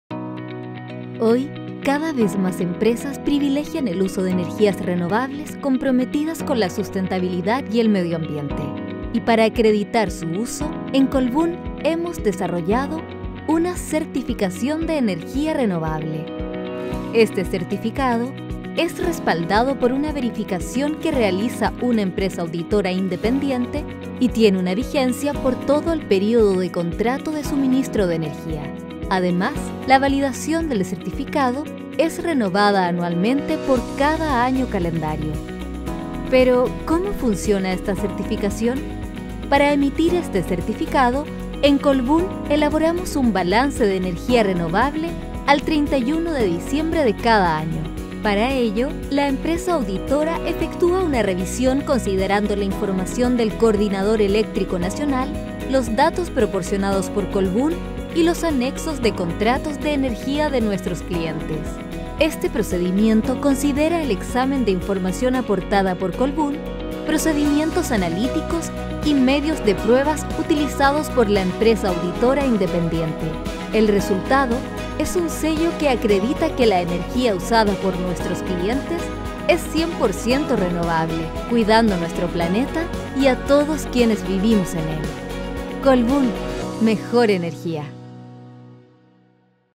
Certificado de Energía Renovable
Este certificado es respaldado por una verificación que realiza una empresa auditora independiente, y tiene una vigencia por todo el periodo de contrato de suministro de energía. ¿Cómo funciona esta certificación? Conoce más en este video.